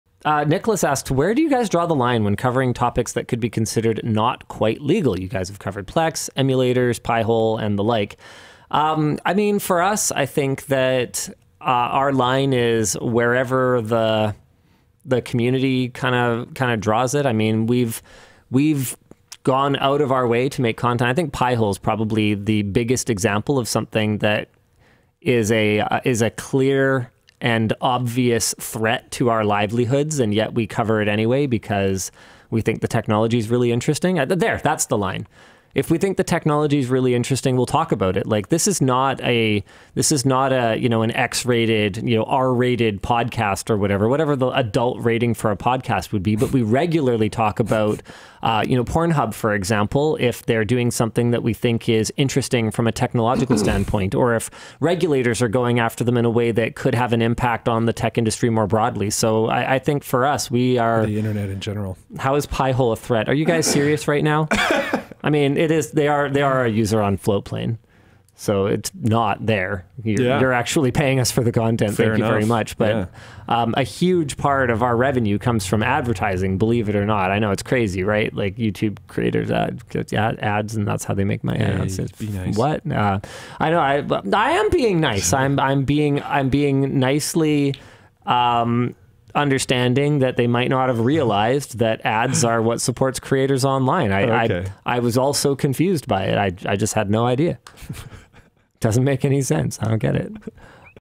Legally Dubious but Fascinating
MM: How does LTT handle covering more legally grey uses of technology, like emulators, Plex, Pi-Hole, etc.